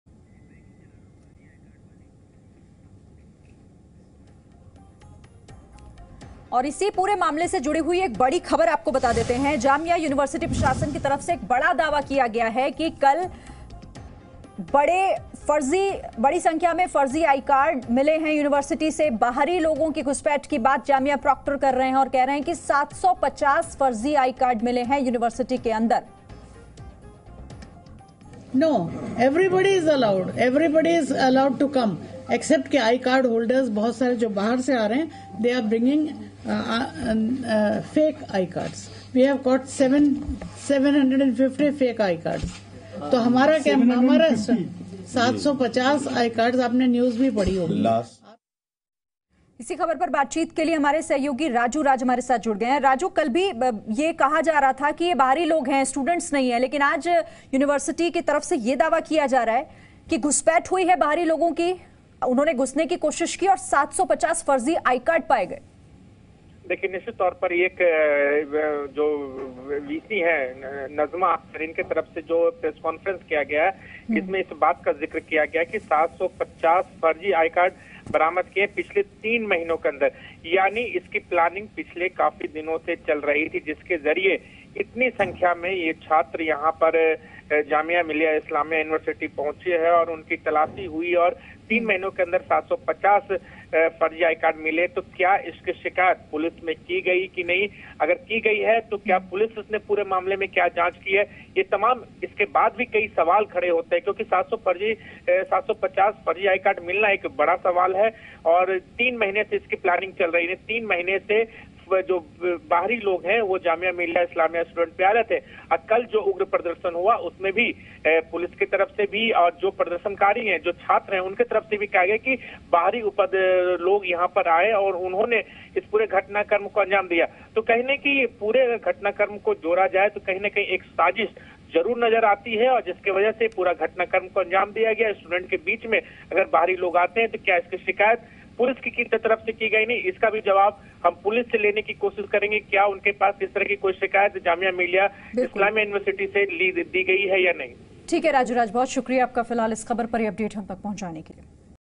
Jamia VC Najma Akhtar claims 750 fake ID cards found inside campus
The students of Delhi's Jamia Millia Islamia University, who were detained by the Delhi Police after a protest march against the citizenship law ended in violence on December 15 have received strong backing from the university's Vice-Chancellor Najma Akhtar. She said that we will demand a high-level probe into the entry of cops inside the campus. The VC also claimed that 750 fake identity cards were found inside the campus.